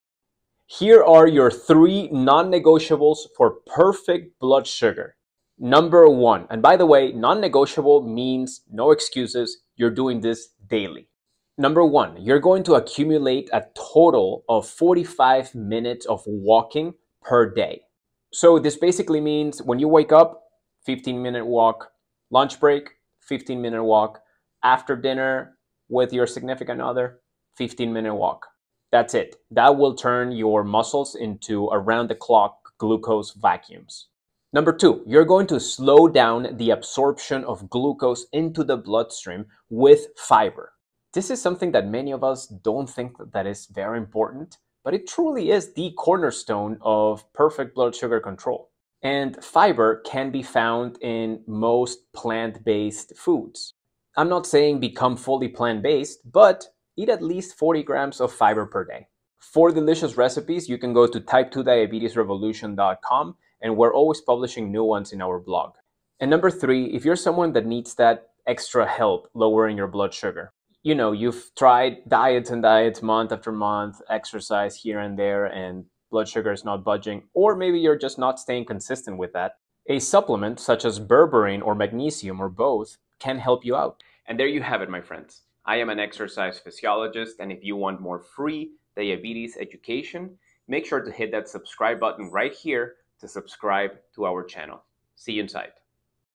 3 Non negotiables for Perfect Blood Sugar
How to lower your A1c before your next doctor’s visit
Want to impress your doctor at your next check-up? Here’s how to lower your A1c naturally using food, movement, and smart habits that actually reverse insulin resistance — not just hide it.

Lower your blood sugar with JADE Pure Berberine (REVOLUTION10)